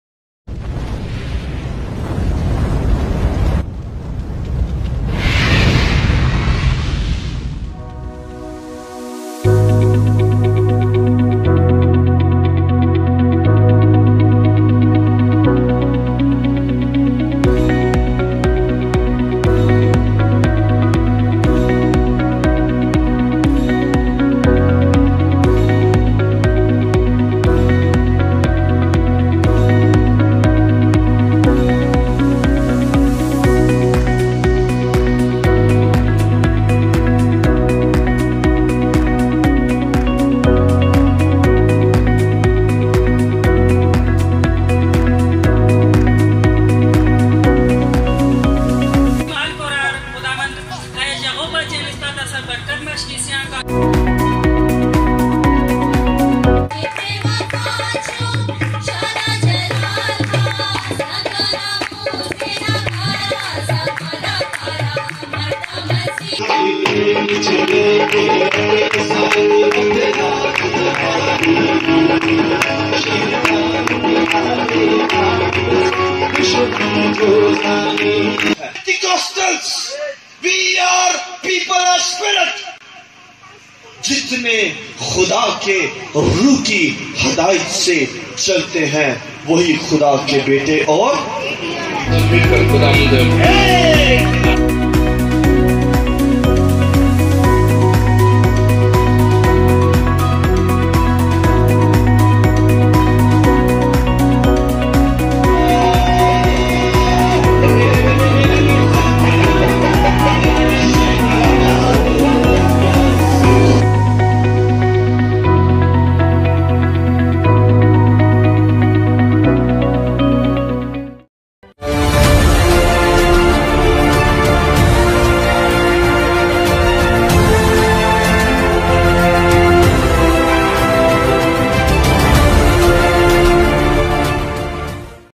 Vision 2020 Tour | Assemblies Of God Church Tangi | Peshawar Kpk
Vision 2020 Tour

Assemblies Of God Church Tangi, Peshawar Kpk,
A branch of mother church (Assemblies Of God Church Peshawar)

We have visited Assemblies of God churches in KPK planted by Rev. Dr. A R Hashmat following the Vision 2020, Working holistically planting churches in major towns and cities of Khyber Pakhtunkhwa inspiring and encouraging our Pastors and Churches to step forward towards the mission of Jesus Christ.